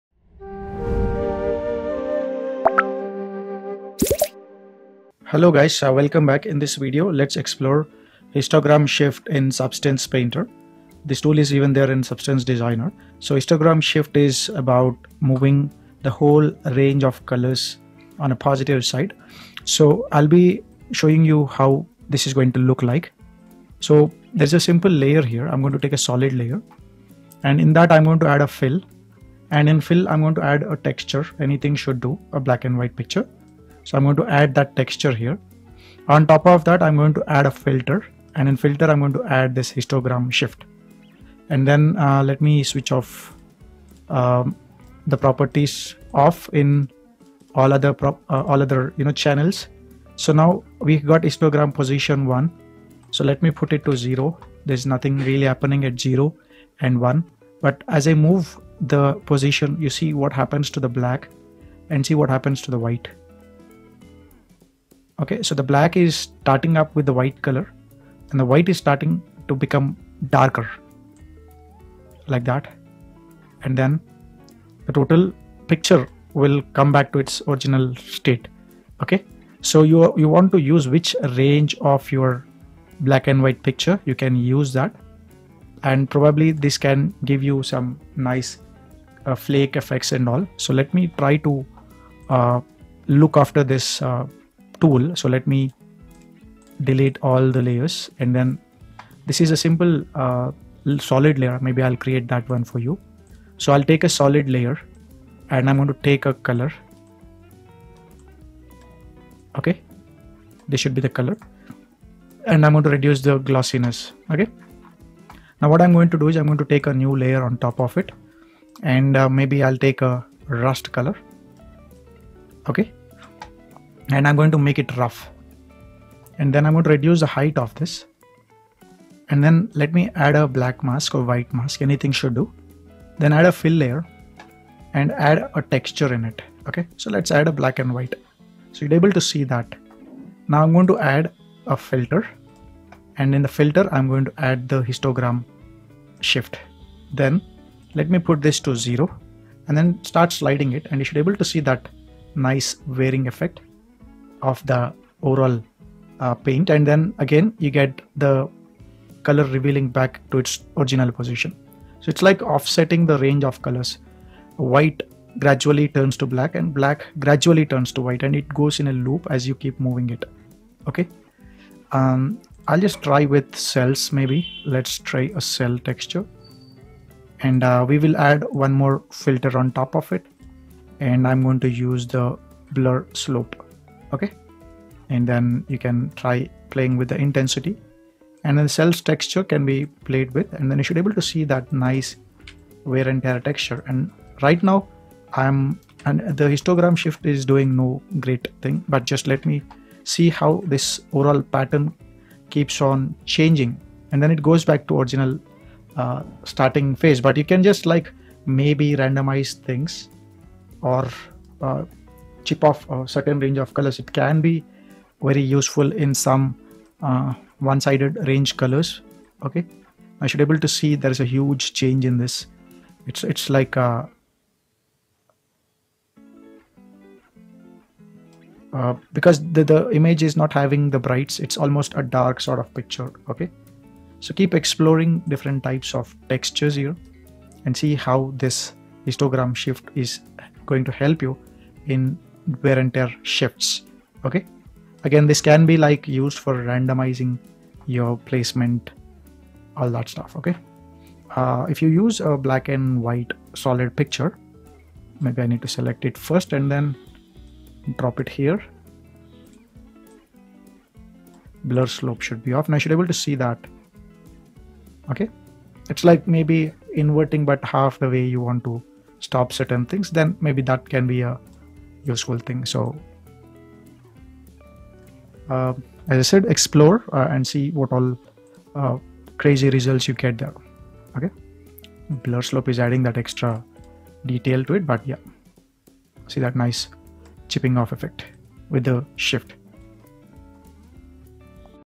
Create Layered Enamel Paint Effects with Histogram Shift & Blur Slope - Substance Painter Tutorial
Welcome to our Substance Painter tutorial where we'll delve into the basics of creating stunning layered enamel paint effects using Histogram Shift and Blur Slope. Whether you're a beginner or an experienced artist, this tutorial will guide you through the process of achieving a realistic and captivating enamel paint finish on your 3D models.

Key Highlights:

Histogram Shift: Learn how to leverage Histogram Shift to control the distribution of color values, adding depth and complexity to your enamel paint layers.

Blur Slope: Explore the power of Blur Slope to simulate the characteristic softening of paint layers based on the underlying surface, achieving a lifelike, dynamic finish.

Layered Enamel Technique: Follow step-by-step instructions on how to layer your enamel paint effects, creating a rich and textured appearance that mimics the real-world nuances of enamel finishes.

Substance Painter Basics: This tutorial is beginner-friendly, making it suitable for those new to Substance Painter. Brush up on essential techniques while creating a visually stunning paint effect.

Unlock the secrets of professional enamel paint rendering in Substance Painter and take your 3D texturing skills to new heights.

Hit the notification bell to stay updated on our latest content, and feel free to leave comments with any specific techniques or effects you'd like us to cover in future tutorials. Let's embark on this creative journey together!